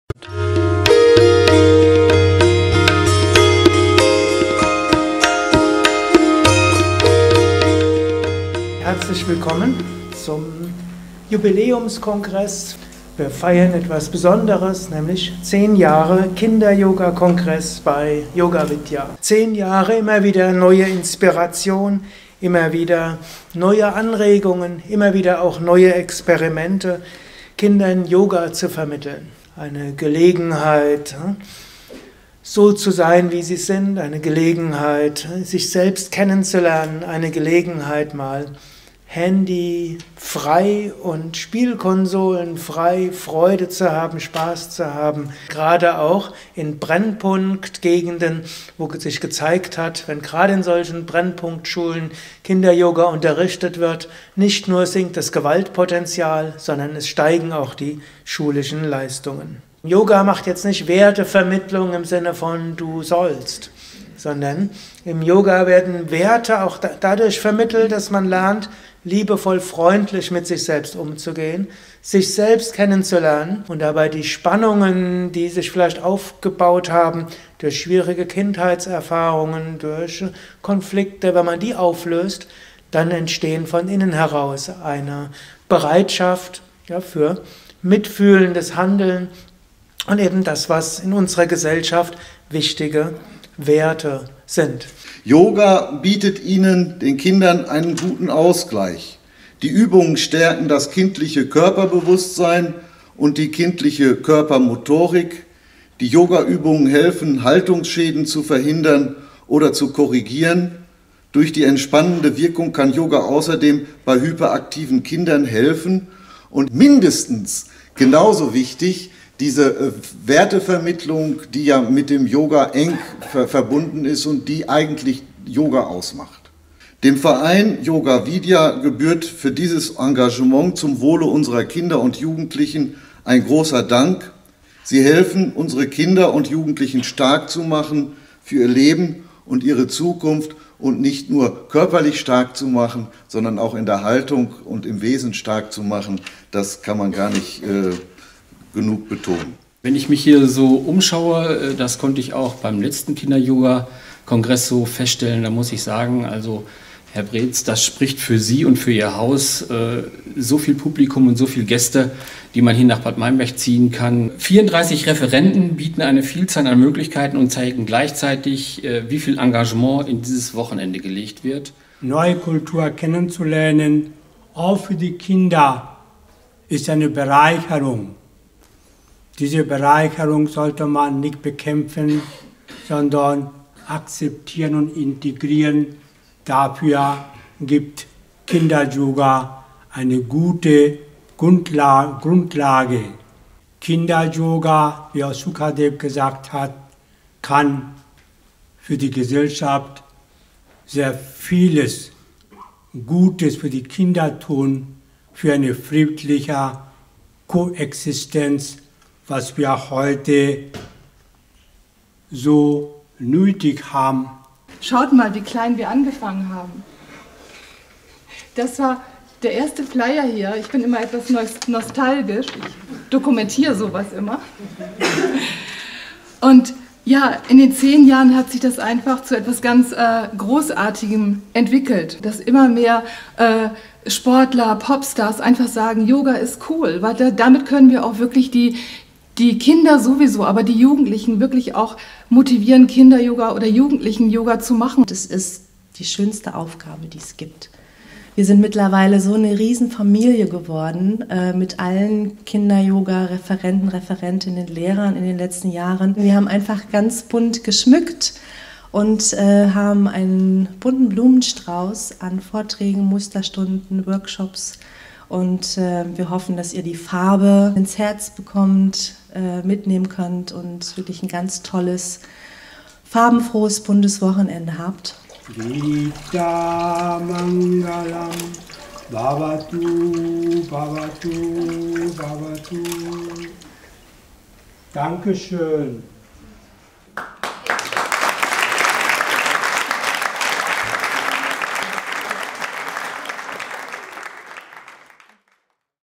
Kinderyogakongress Eröffnung 2015 - Highlights der Eröffnungsfeier
„Mit Kindern gemeinsam Werte entdecken“ lautete das Motto des 10 jährigen Kinderyoga-Jubiläums-Kongress von Yoga Vidya in Bad Meinberg. Wir haben ein umfangreiches Seminarangebot über Kinder und Yoga Hier ist eine Zusammenfassung des Live-Mitschnitts der feierlichen Eröffnung des Kinderyoga-Kongresses am 6.März 2015 bei Yoga Vidya. Kindern Yoga zu vermitteln, ihnen neue Erfahrungen ermöglichen und Spaß und Freude zu geben. Gelebte menschliche Werte wie Dankbarkeit, Wahrheit, Gewaltlosigkeit, Liebe und Wertschätzung in der Familie und der Gesellschaft unterstützen Kinder und Jugendliche in ihrer Entwicklung. Yoga kann Quelle zur Förderung der Konzentration, der Selbstwahrnehmung und Entspannung, der Haltungs- und Willensstärkung, des Durchhaltevermögens und Ausdrucks der eigenen Gefühle sein.